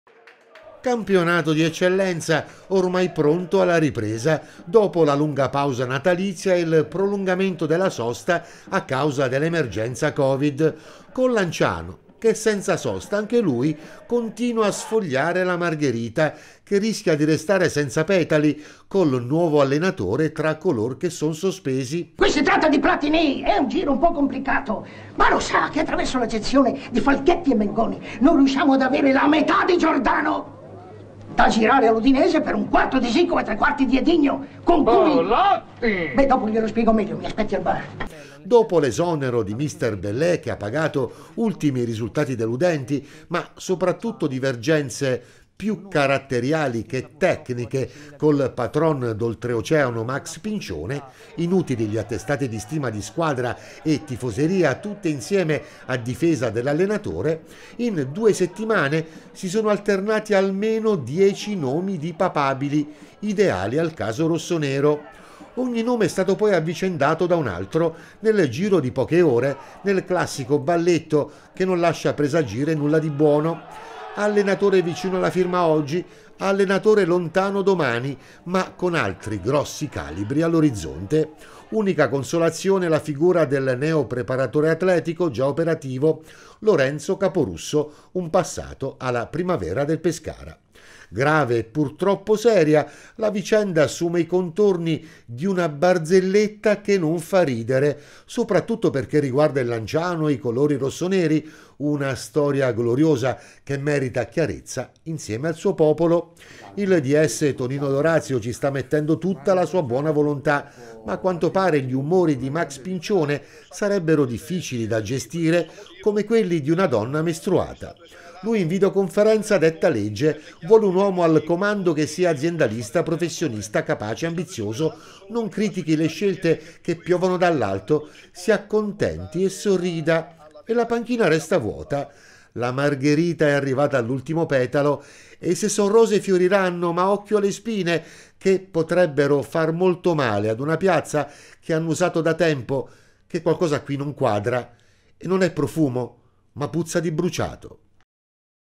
Lanciano calcio, allenatore cercasi
Eccellenza: il Lanciano resta ancora senza allenatore, dopo decine di contatti e nessuna ufficialità a pochi giorni dalla ripresa del campionato.